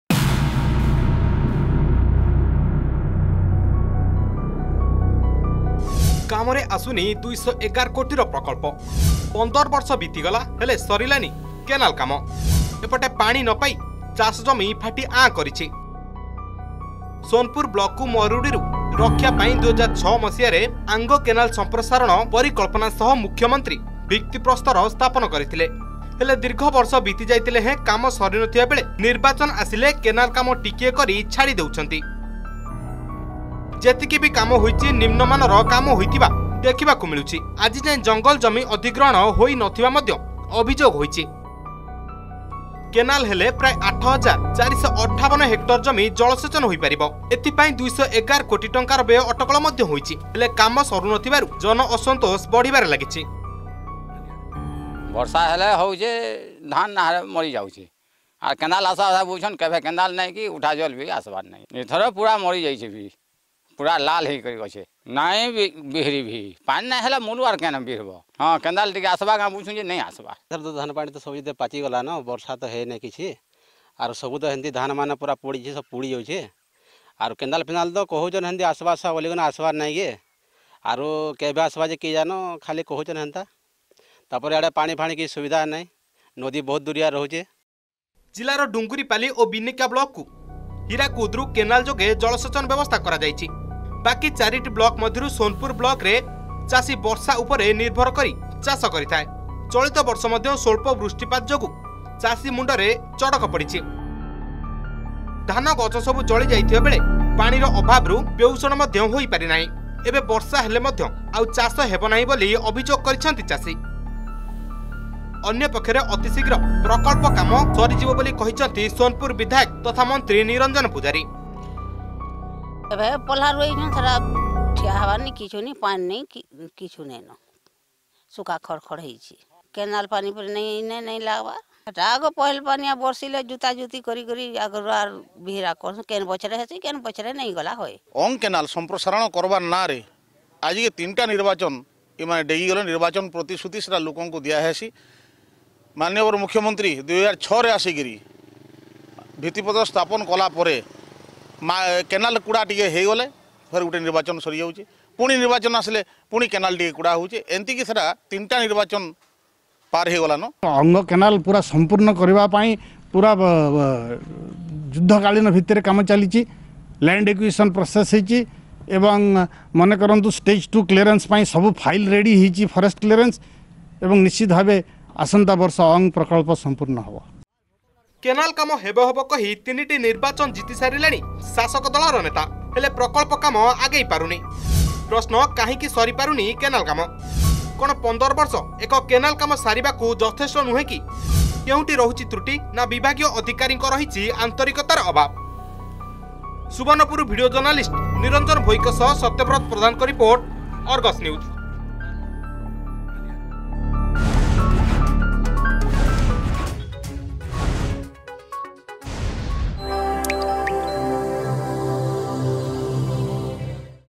କାମରେ ଆସୁନି ୨୧୧ କୋଟିର ପ୍ରକଳ୍ପ
ସୋନପୁର ବ୍ଲକକୁ ମରୁଡିରୁ ରକ୍ଷା ପାଇଁ ୨୦୦୬ ମସିହାରେ ଆଙ୍ଗ କେନାଲ ସଂପ୍ରସାରଣ ପରିକଳ୍ପନା ସହ ମୁଖ୍ୟମନ୍ତ୍ରୀ ଭିତ୍ତିପ୍ରସ୍ତର ସ୍ଥାପନ କରିଥିଲେ । ହେଲେ ଦୀର୍ଘ ବର୍ଷ ବିତିଯାଇଥିଲେ ହେଁ କାମ ସରିନଥିବାବେଳେ ନିର୍ବାଚନ ଆସିଲେ କେନାଲ କାମ ଟିକିଏ କରି ଛାଡି ଦେଉଛନ୍ତି । ଯେତିକି ବି କାମ ହେଉଛି ନିମ୍ନମାନର କାମ ହେଉଥିବା ଦେଖିବାକୁ ମିଳିଛି ।